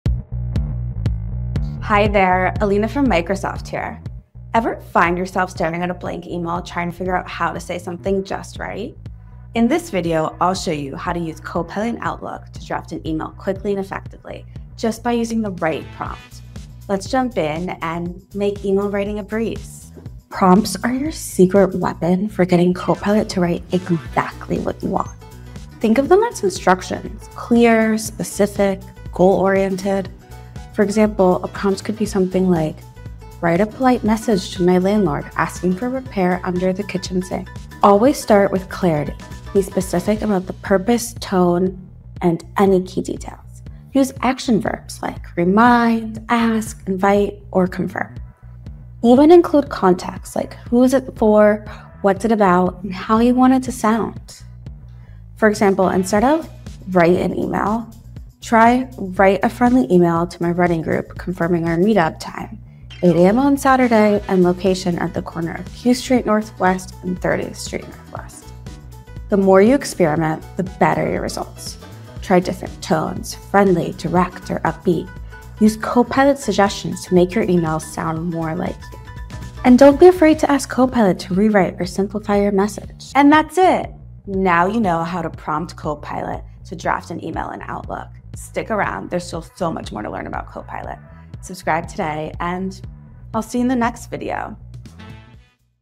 How to Prompt: Drafting Emails
Ever find yourself staring at a blank email draft, knowing what you need to say but struggling to write it out? You're not alone--and there's an AI tool to help you write, edit, and refine your emails that lives right in Outlook. If you need a little help starting an email draft, Copilot in Outlook can help. Give it clear, specific instructions, and you'll have a draft to work with in seconds.

What you'll learn:
• How to generate an email draft in Outlook
• How to optimize your Copilot email prompts

Timecodes:
00:26 - How to structure a good prompt
00:32 - Example prompt for Copilot in Outlook
00:49 - How to give Copilot the right context in your prompt
00:55 - Example email prompt: Details and context in action
01:19 - How to rewrite and edit your email with Copilot